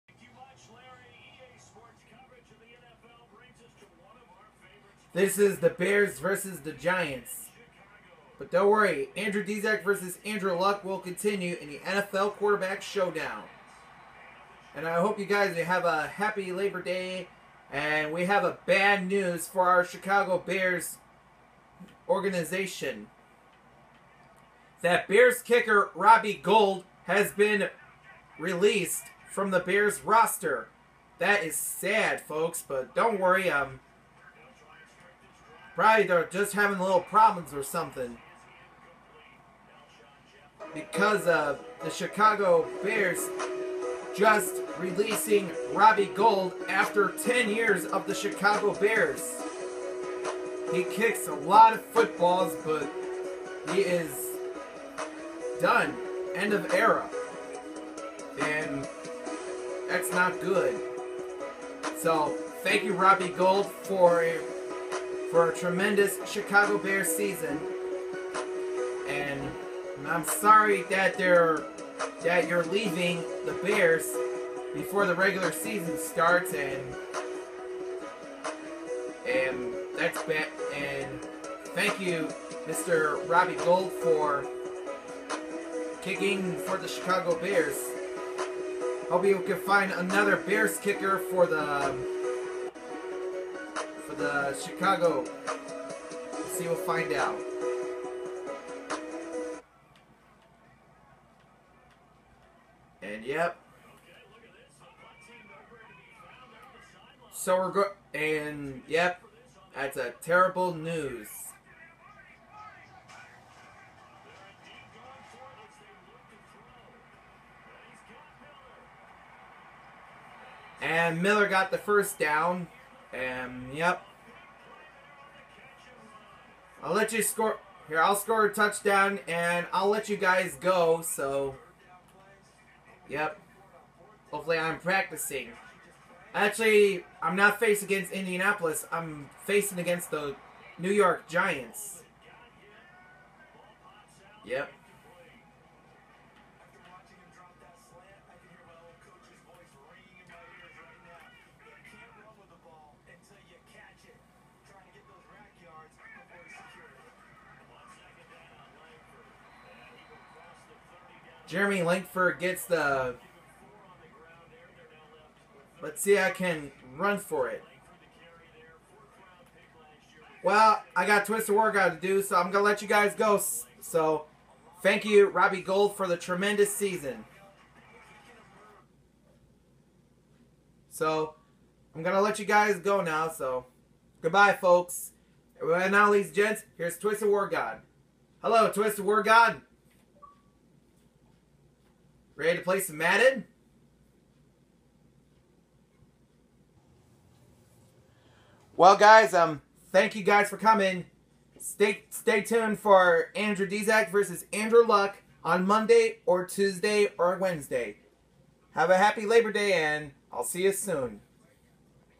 Robbie Gould - End of ERA
Kicker Robbie Gould is no longer being a Chicago Bears he's been released